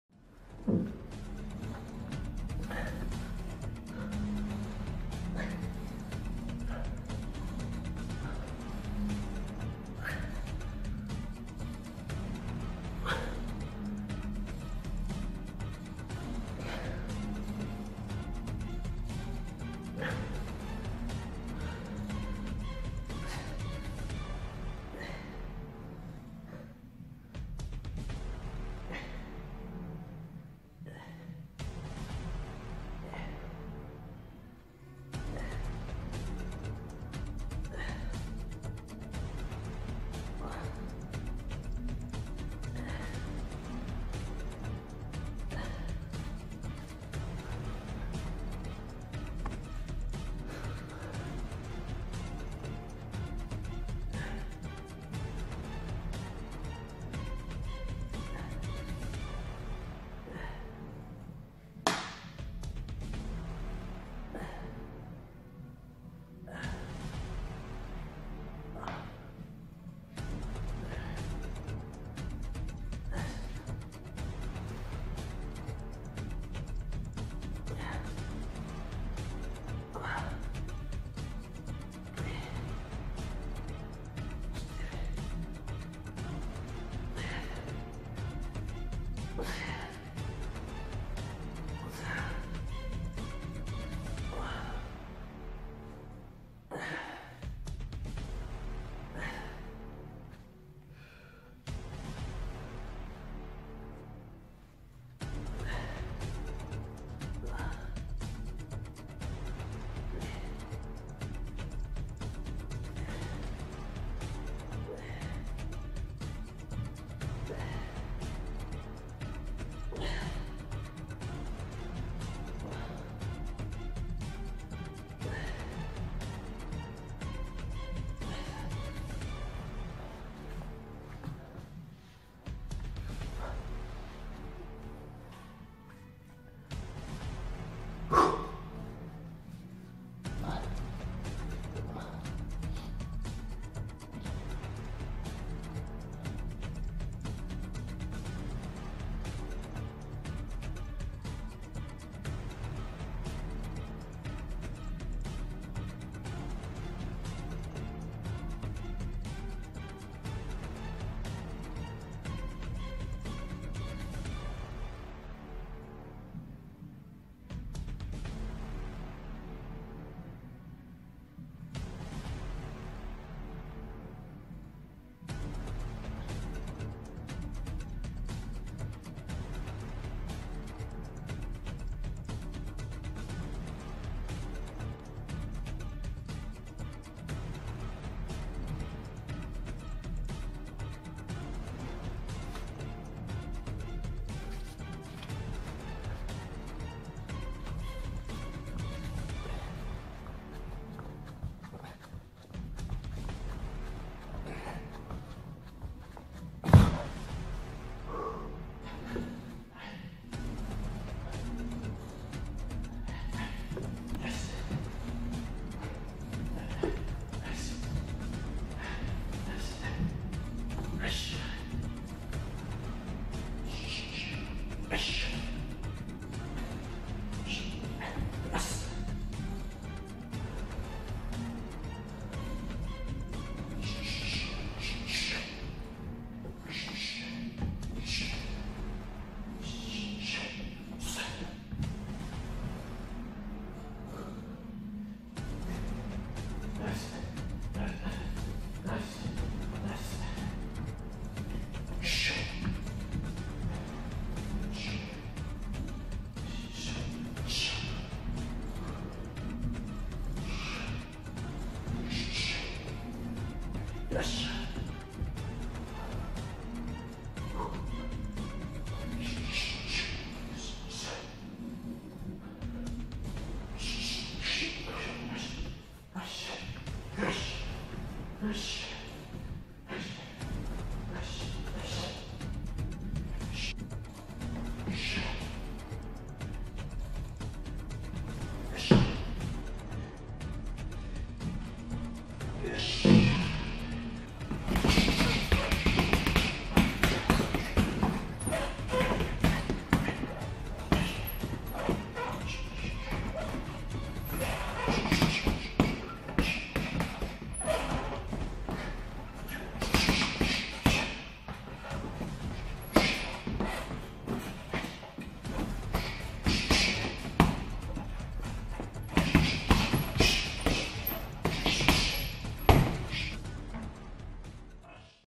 Working out
Mein Video beim trainieren.Ich weiss der Name passt nicht wirklich dahinn,aber seht darüber weg.)=)